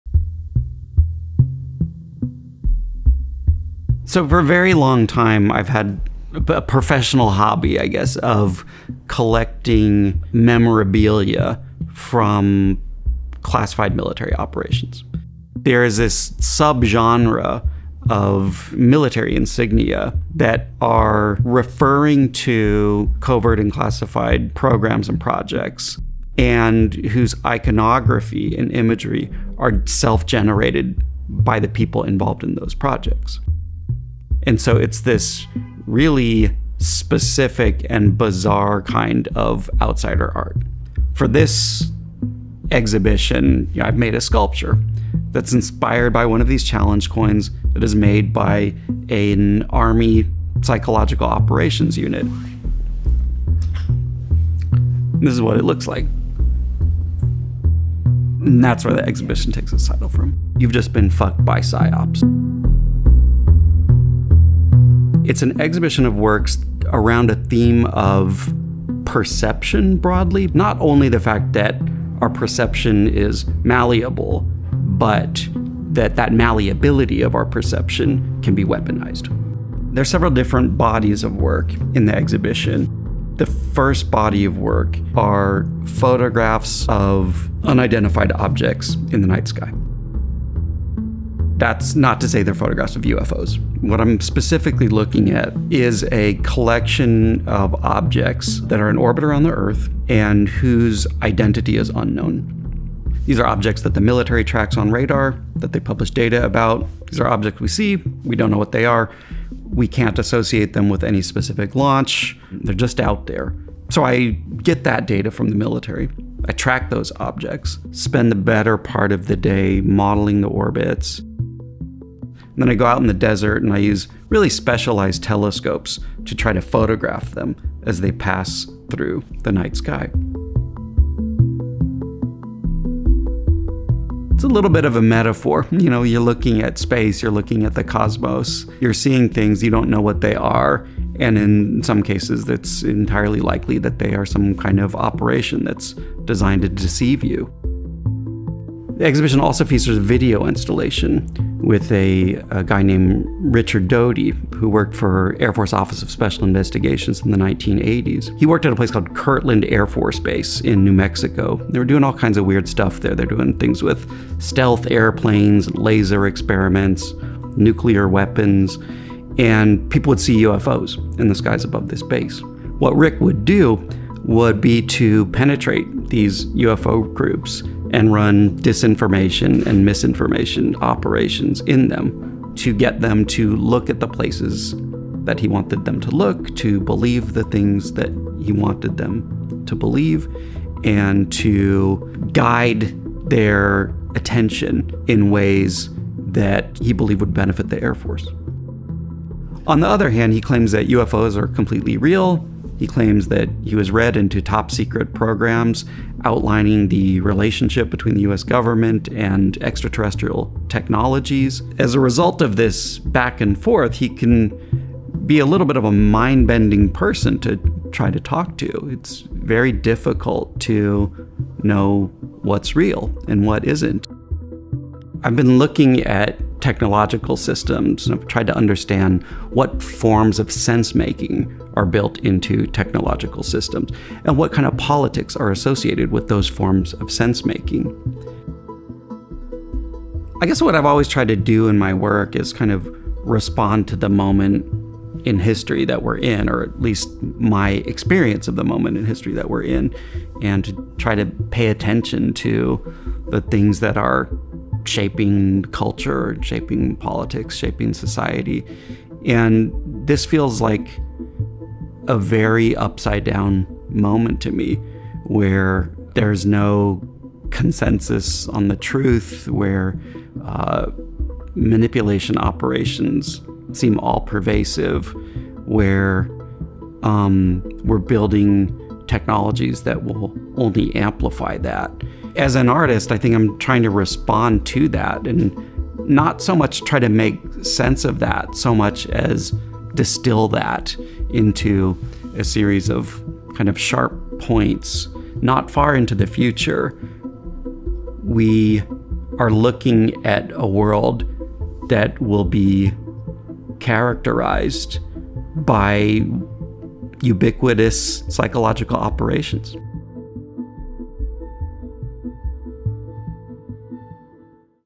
Trevor Paglen’s Known Unknowns
In a new film, Trevor Paglen parses issues of subjectivity and deception that surface in his photographs of “unids”—unknown objects in orbit around the Earth that are monitored and tracked by the US military—new video installation spotlighting counterintelligence official Richard Doty, and a new large-scale sculpture inspired by the “challenge coin” used by the US army’s Military Information Support Operations (MISO) command. These bodies of work will be featured as part of the artist’s first solo presentation with our gallery in New York from May 12 to July 22, 2023.